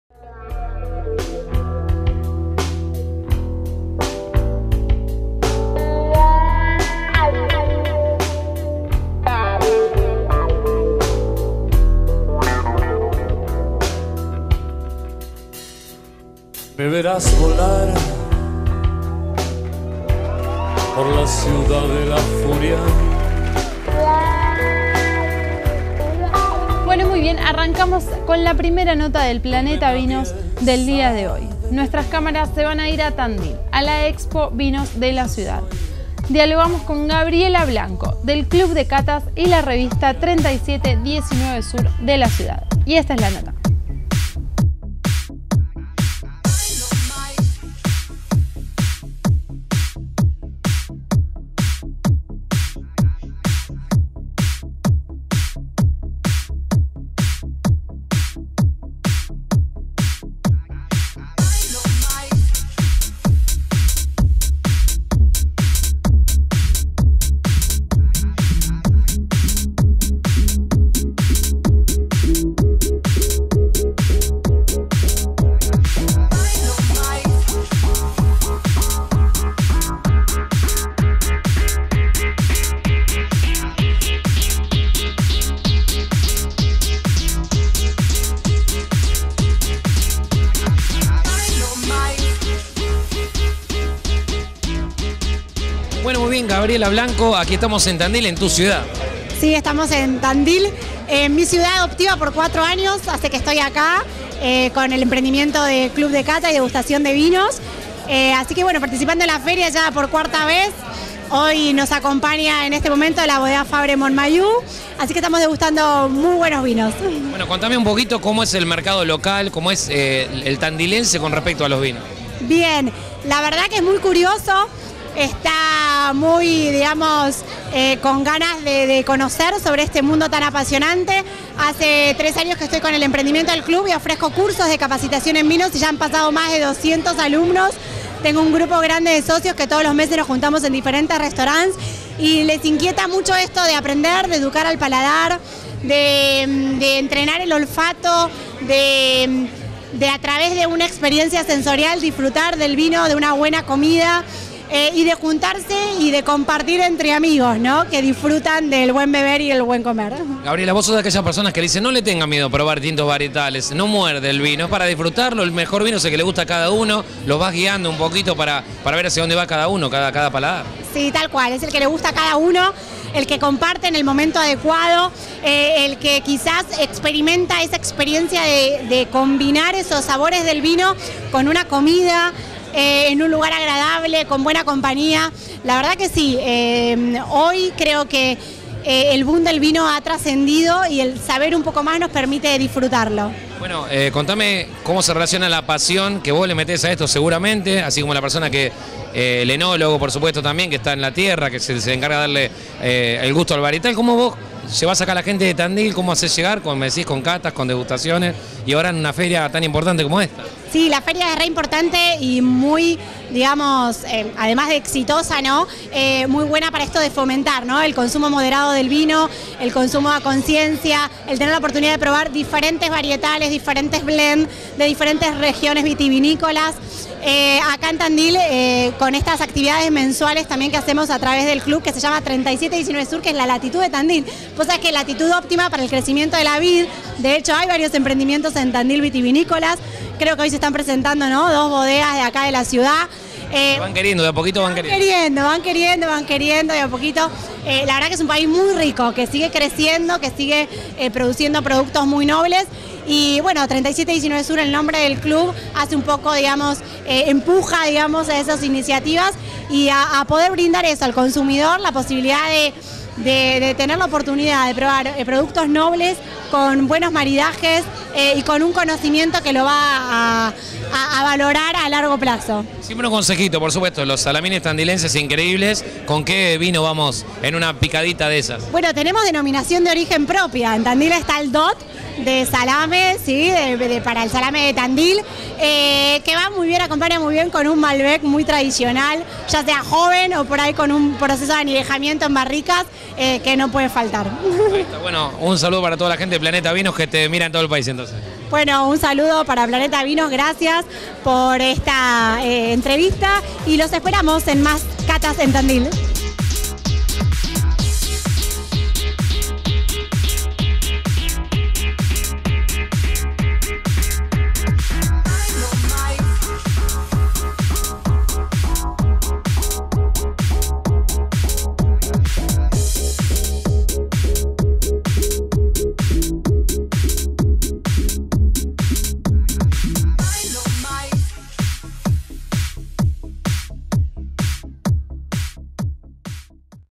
Expovinos Tandil Nota a Gabriela Blanco
PLANETA VINOS sigue presentando a toda la Argentina notas a los protagonistas del mundo del Vino. Gabriela Blanco es Directora de la revista 37 19 sur y de Club de Cata, desde Tandil a todo el mundo